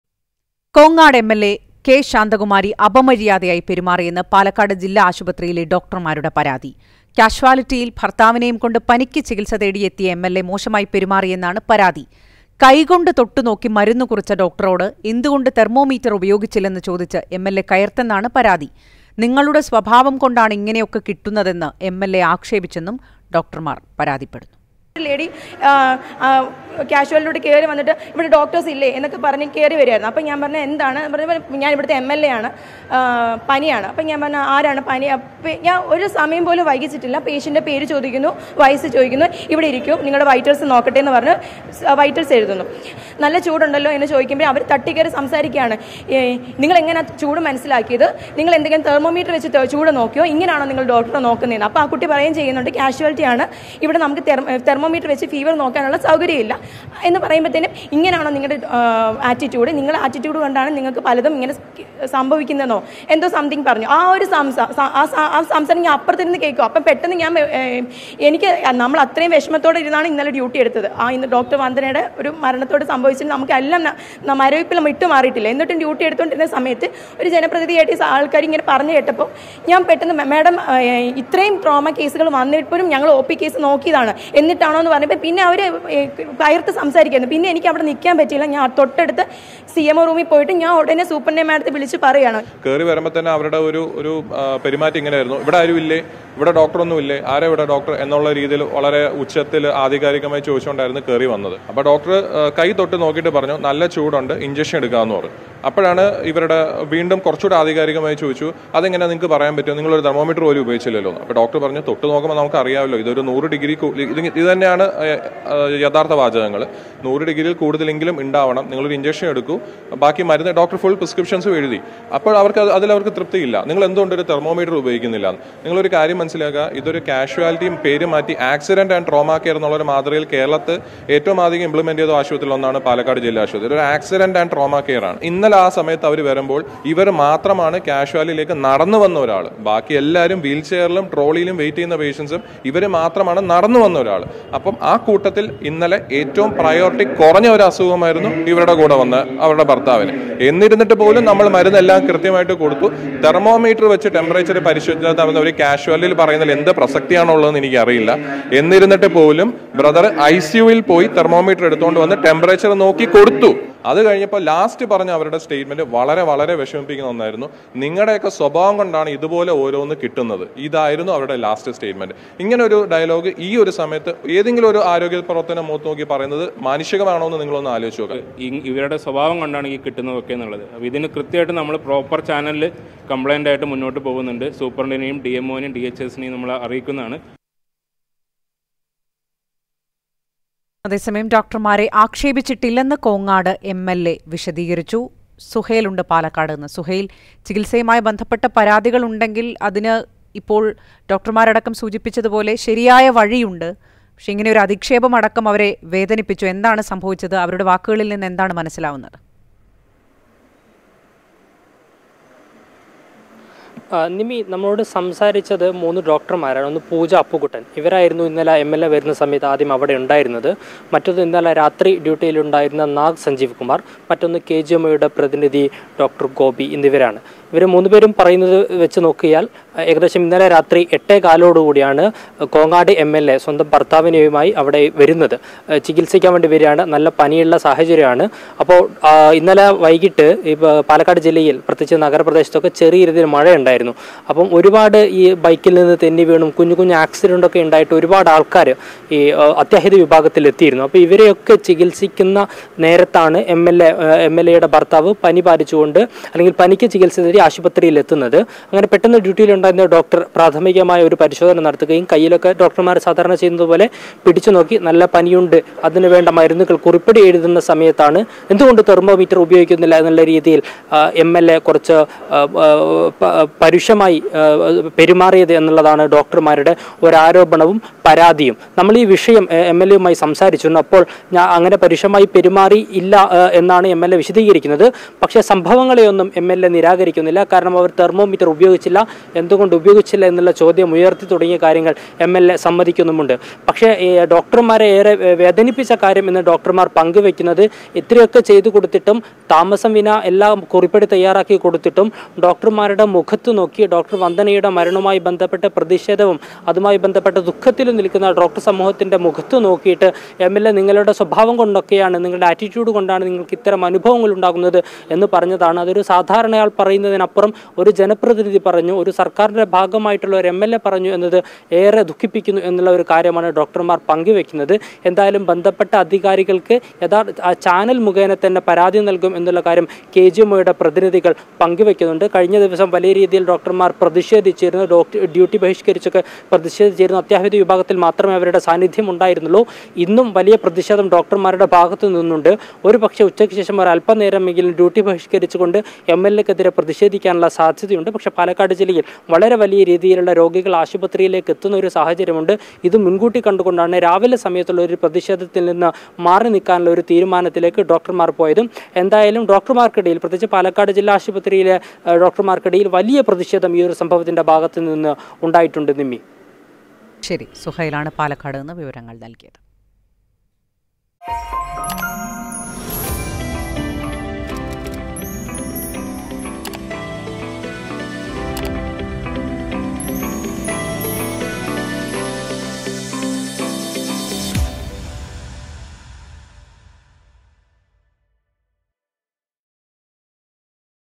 തെർമോമീറ്റർ ഉപയോഗിക്കാത്തതിന് ഡോക്ടർമാരോട് അപമര്യാദയായി പെരുമാറി കോങ്ങാട് എംഎൽഎ |K.Santhakumari mla
'നല്ല ചൂടുണ്ടല്ലോ എന്ന് പറഞ്ഞതോടെ തെർമോമീറ്ററില്ലാതെ എങ്ങനെ ചൂടറിഞ്ഞു എന്ന് ചോദിച്ച് തട്ടിക്കയറി, നിങ്ങളുടെയൊക്കെ സ്വഭാവം കൊണ്ടാണ് ഓരോന്ന് കിട്ടുന്നതെന്ന് പറഞ്ഞു', ഭർത്താവിനെ ചികിൽസിച്ച ഡോക്ടർമാരോട് അപമര്യാദയായി പെരുമാറി കോങ്ങാട് എംഎൽഎ കെ ശാന്തകുമാരി
K.Santhakumari mla | Doctors